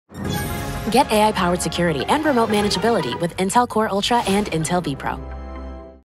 Intel Core Ultra processors and Intel vPro — Clones | Intel Business
Power up and manage PCs remotely. Plus, get AI-powered threat detection for better security. No clones needed when you have Intel Core Ultra processors and Intel vPro.